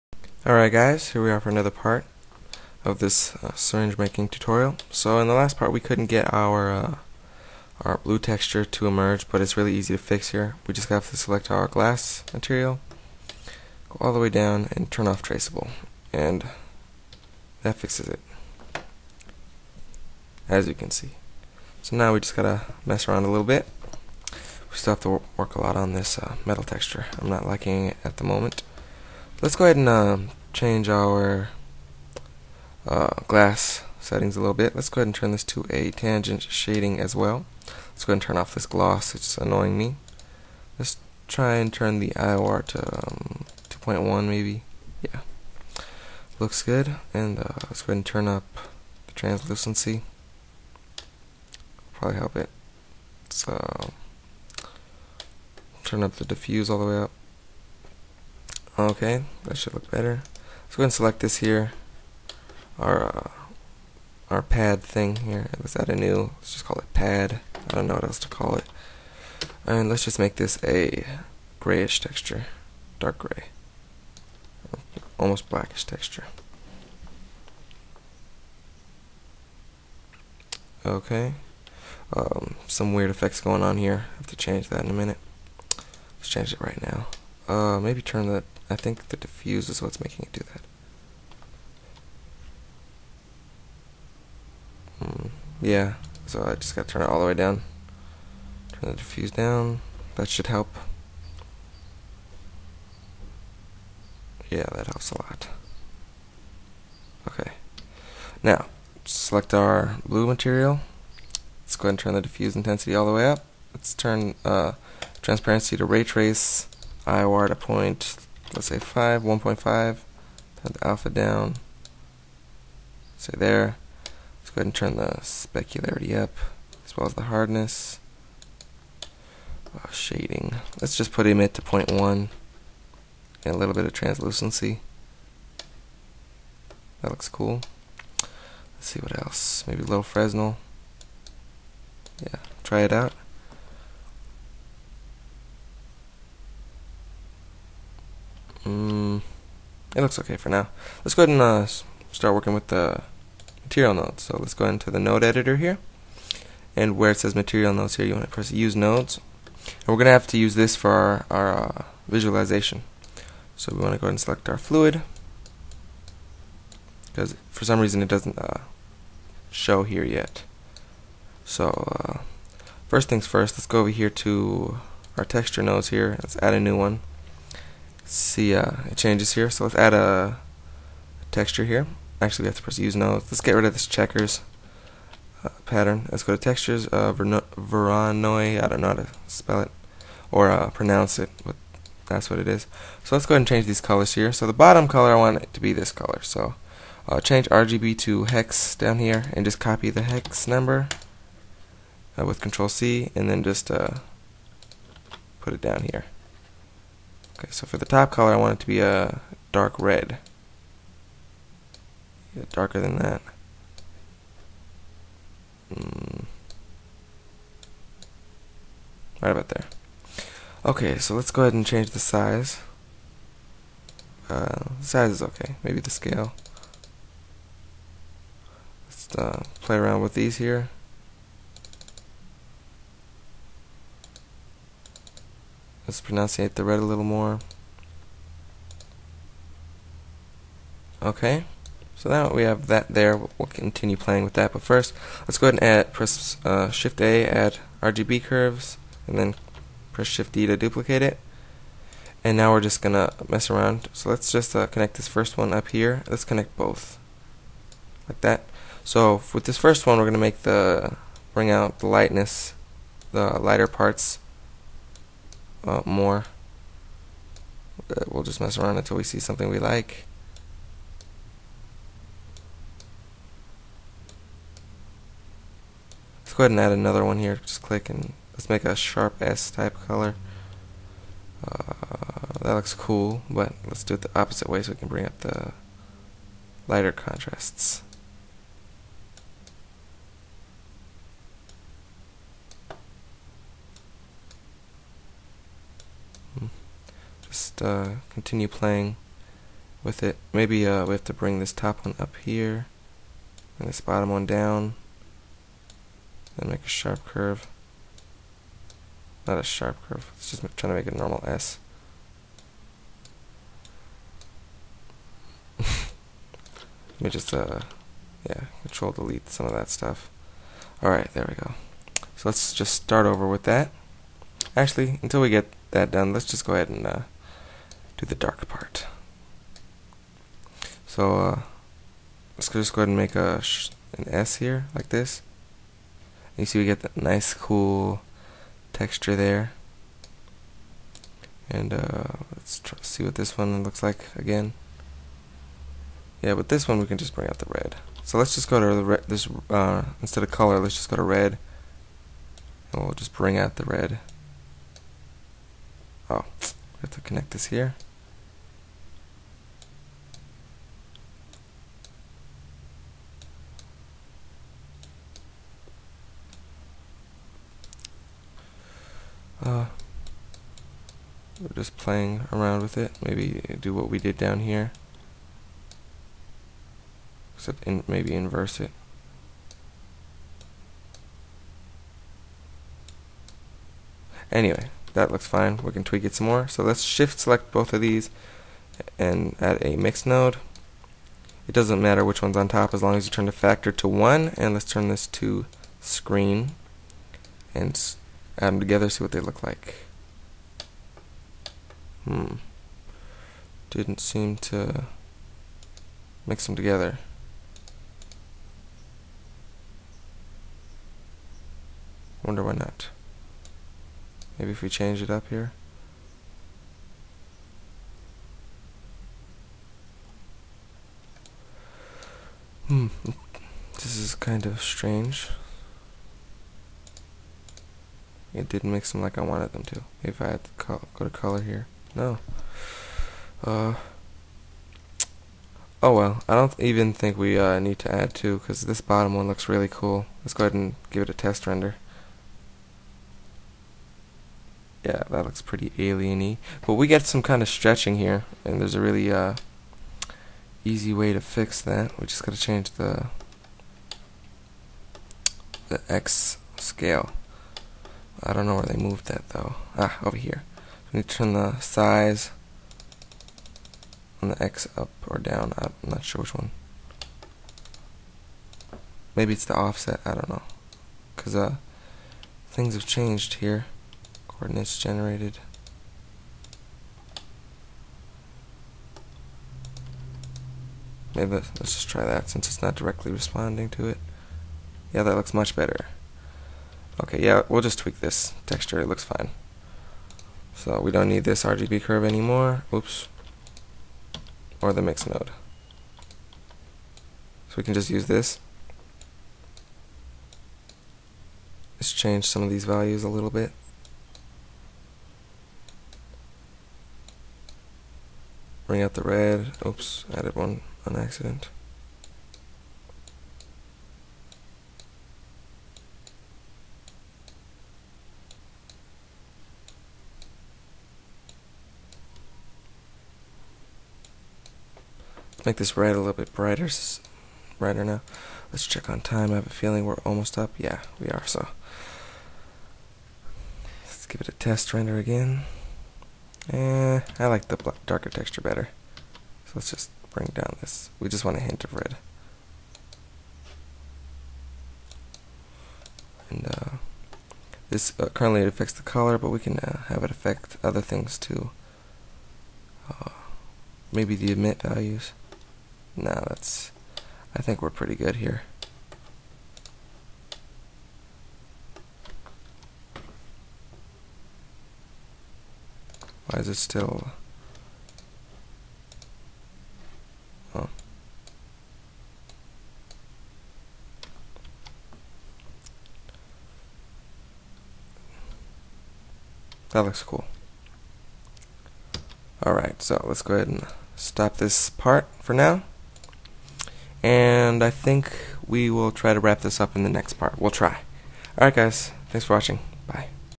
Blender Tutorial - Vintage Syringe Part 6/7
This is the sixth of seven parts on making a vintage syringe in Blender 2.5.